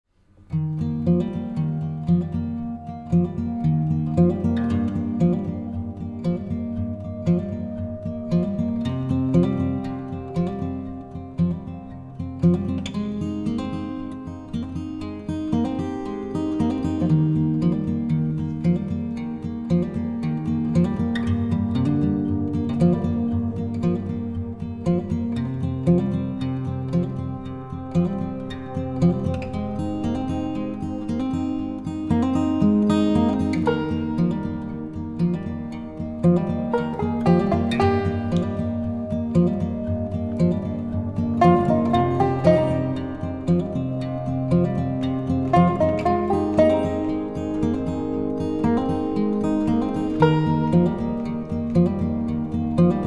Season's first snow in time of Halloween - Chicago north suburbs
Season's first snow in time of Halloween - Chicago north suburbs. trick or treat would need a boot this time